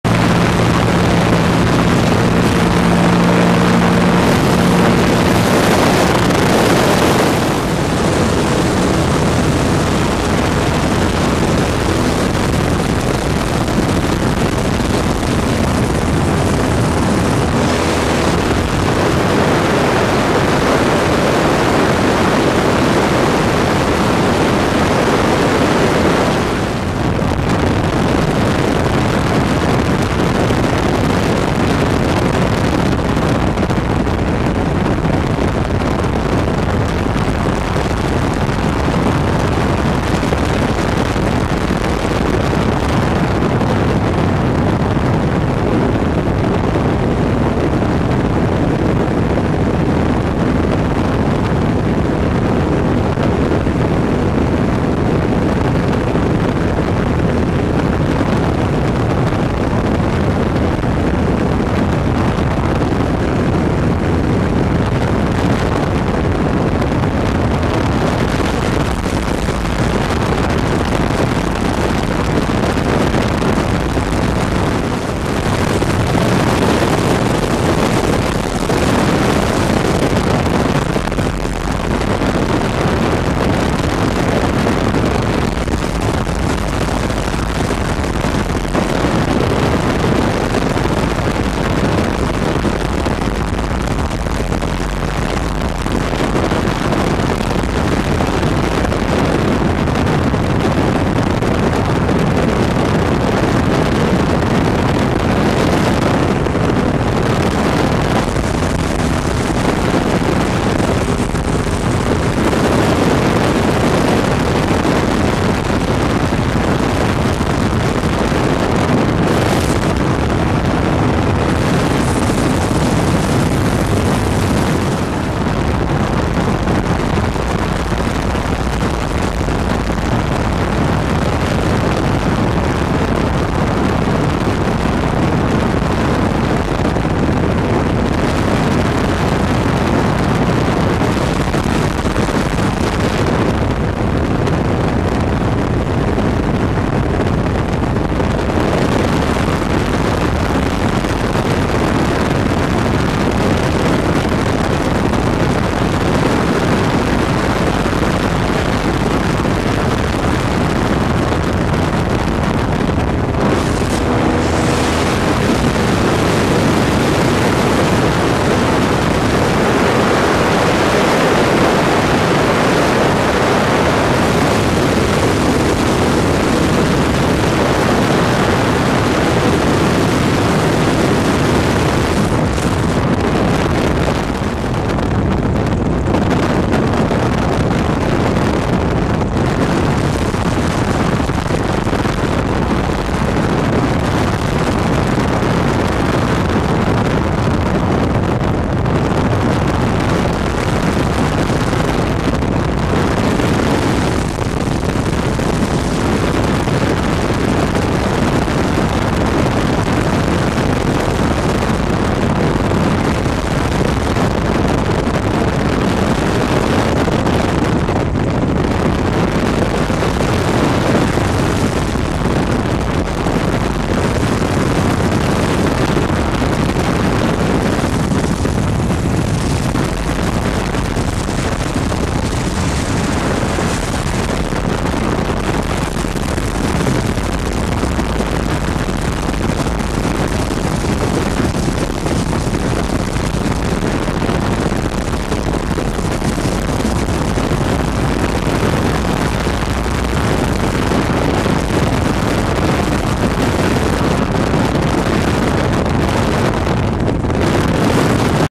ulm en guyane
voici un petit tour d'ulm pour découvrir la guyane du ciel